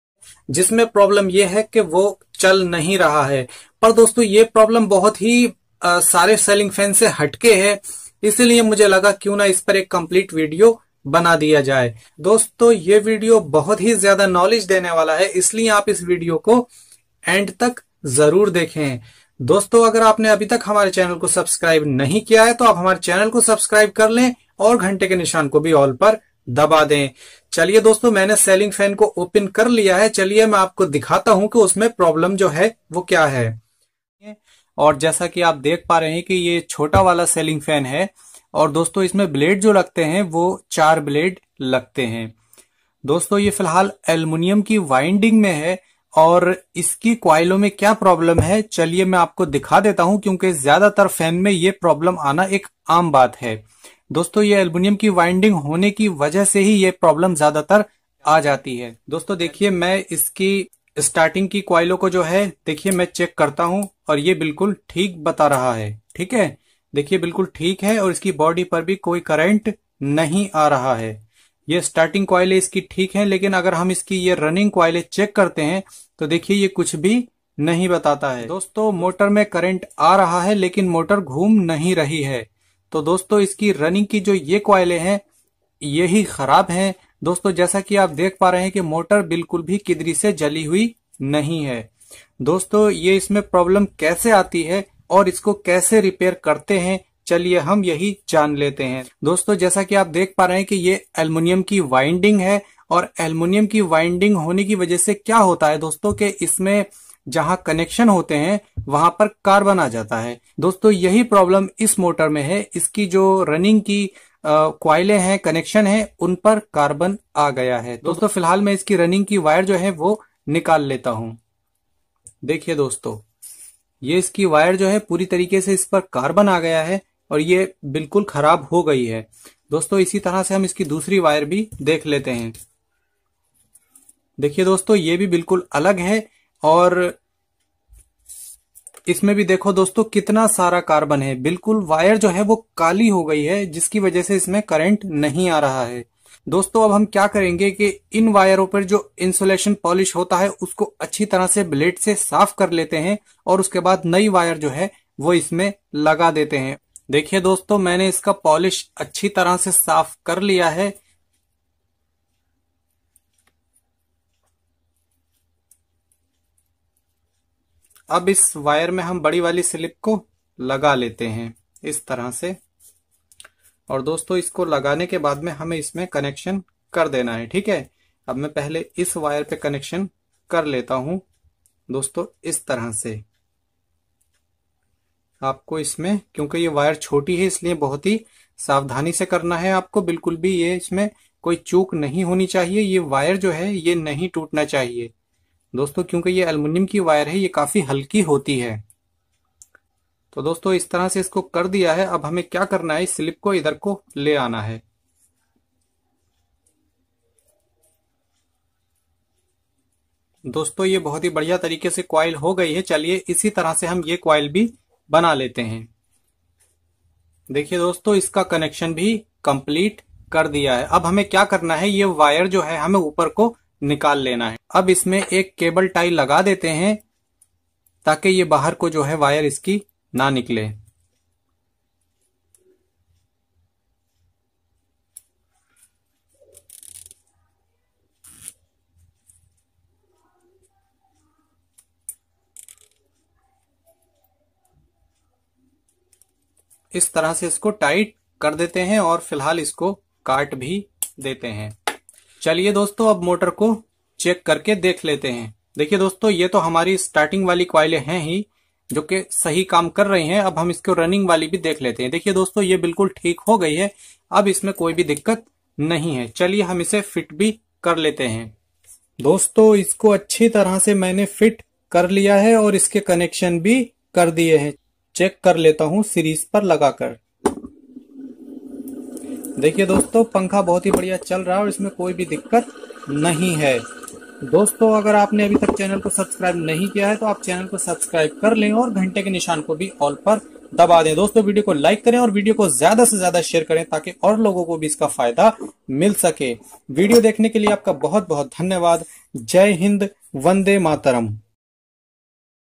capacitor change karke bhi chalta nahin hai pankha | fan running slow | how to increase fan speed
title

about
doston repairing channel Mein aapka swagat hai aaj main aapke liye ceiling fan ka video Lekar Aaya hun aur is ceiling fan may problem Yeh hai ki iska capacitor change karke bhi fan slow chalta hai to ismein kya problem hoti hai aur isko Kaise repair Karenge yahi sab aaj main aapko is video mein batane wala hun isliye video pura Dekhen Pasand Aaye like Karen doston ke sath share Karen thank u